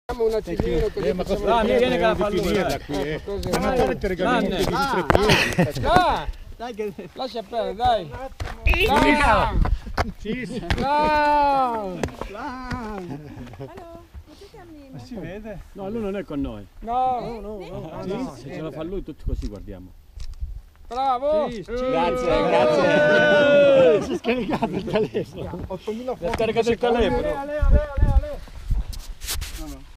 #146_26.06.2016 Alpe d'Huex, Col de Sarenne, Les deux Alpes-GOPR2398 gruppo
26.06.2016 - Alpe d'Huex, Col de Sarenne, Les deux Alpes: 75,7 km, Tempo: 4h 23’ Velocità media: 17,3 km/h, Aumento di quota: 2.195m, Calorie 2.561 C, Temperatura media: 22,0 °C, Pot. media 145W, Vel. Max 98,6 Km/h
Telecamera:GoPro Hero3+ e GoPro 4 con Garmin Edge 810

PER UNA VISUALIZZAZIONE OTTIMALE, SETTATE LA QUALITA' DEL VIDEO A 1080p